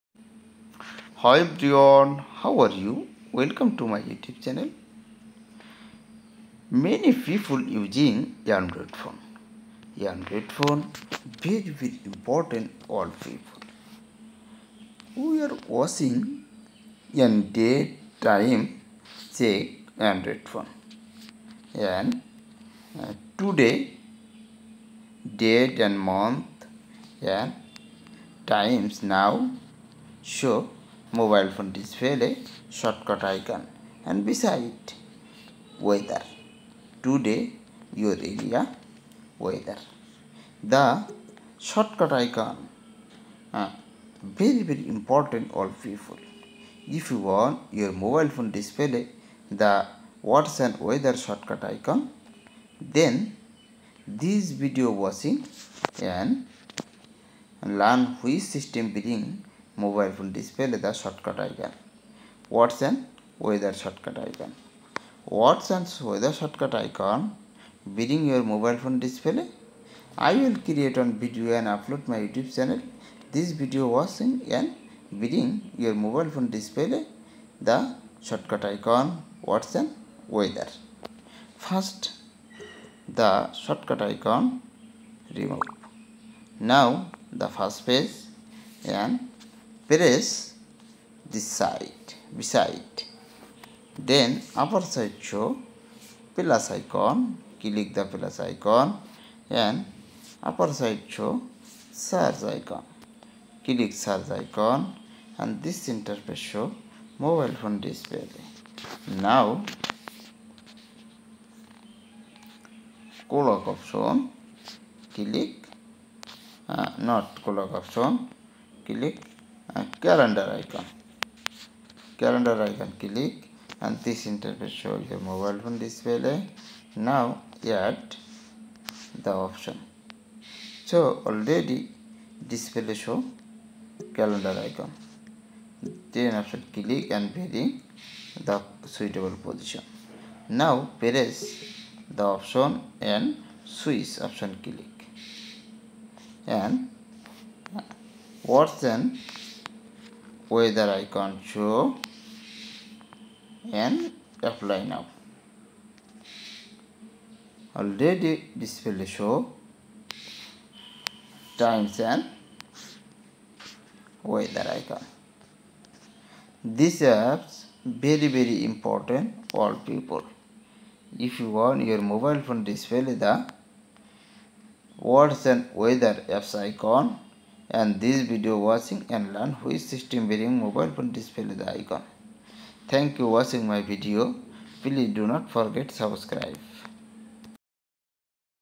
Check time and weather in a shortcut app
This video name is Check time and weather in a shortcut app. This video name is very important for Android phone user people and also important for all mobile phone user people.

Clock shortcut icon bring on display
Weather shortcut icon bring on display
Clock and weather shortcut icon bring on display